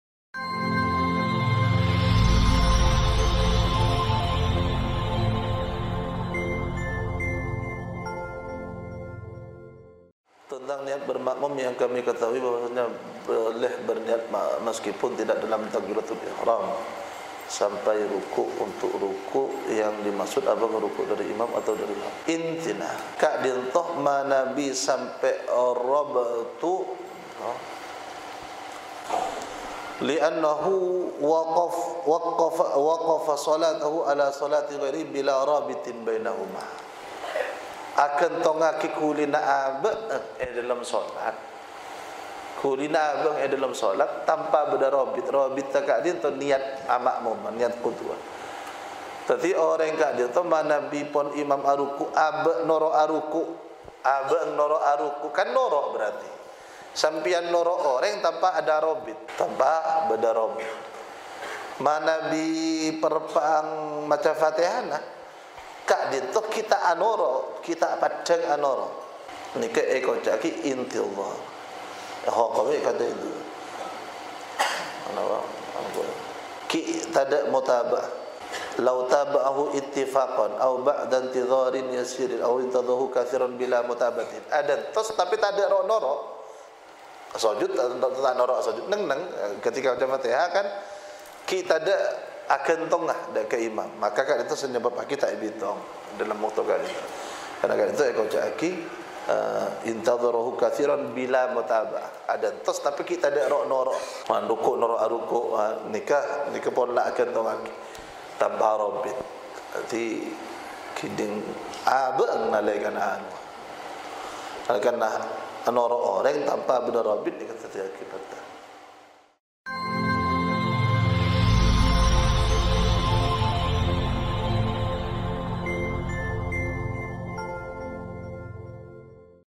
Membaca Niat Di Dalam /Sebelum Takbiratul Ihrom Sah? II Abuya Abd Latif Adzim El AtiQ,.
Video ini adaah jawaban dari pertanyaanapa yang harus  dilakukan ma'mum yang ketika ingin rukuk ternya di depannya tiba tiba ada sesuatu yang menghalang semisal ada anak kecil, sedangkan ma'mum untuk bergerak kepinggir atau kebelakang tidak bisa disebabkan ada ma'mum yang lain dan jika tidak mengikuti imam itu hukumnya dapat membatalkan sholat.
Lalu apa yg harus dilakukan oleh ma'mum tersebut. ?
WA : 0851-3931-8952